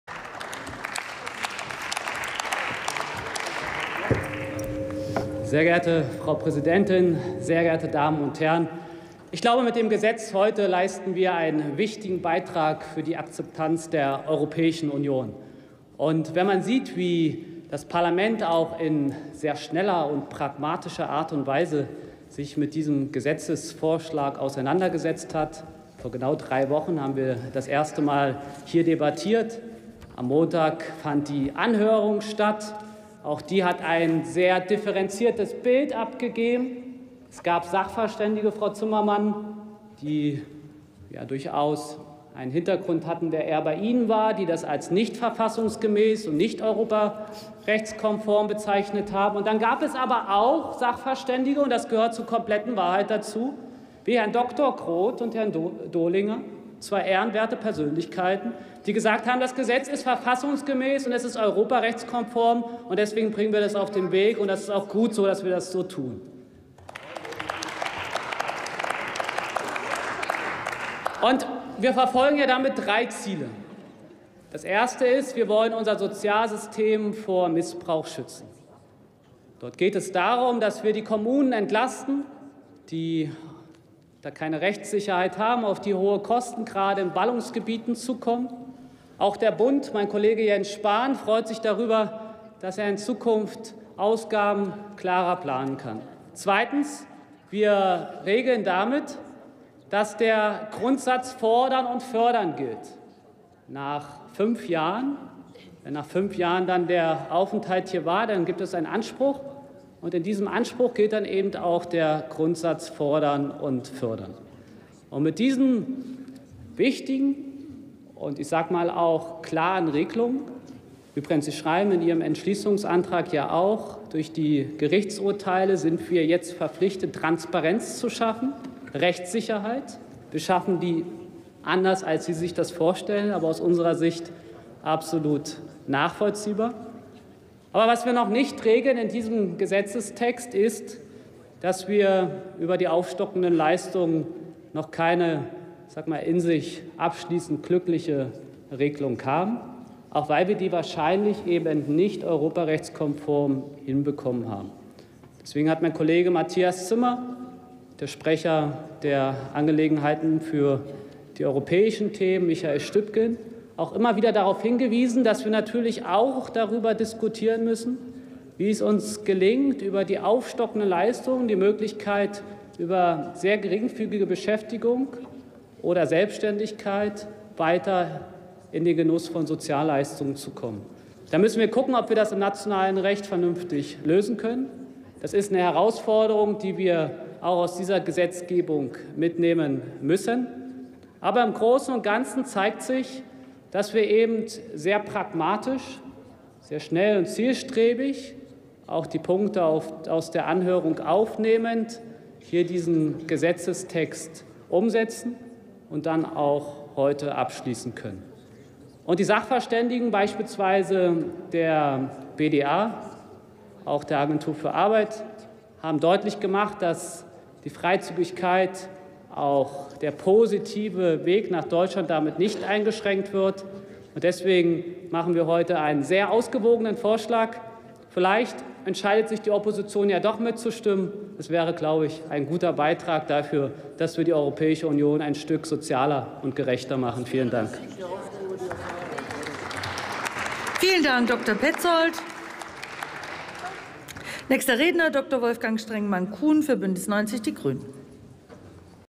Martin Pätzold: Grundsicherung für arbeitsuchende Ausländer [Bundestag 01.12.2016]
eingebrachten Entwurfs eines Gesetzes zur Regelung von Ansprüchen ausländischer Personen in der Grundsicherung für Arbeitsuchende nach dem Zweiten Buch Sozialgesetzbuch und in der Sozialhilfe nach dem Zwölften Buch Sozialgesetzbuch
Drucksache 18/10211
Beschlussempfehlung und Bericht des Ausschusses für Arbeit und Soziales (11. Ausschuss)
Drucksache 18/10518

18. Deutscher Bundestag
206. Plenarsitzung
Tagesordnungspunkt: Grundsicherung für arbeitsuchende Ausländer
Redner: Pätzold, Prof. Dr. Martin, CDU/CSU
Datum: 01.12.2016
Plenarprotokoll:
UVID:7039024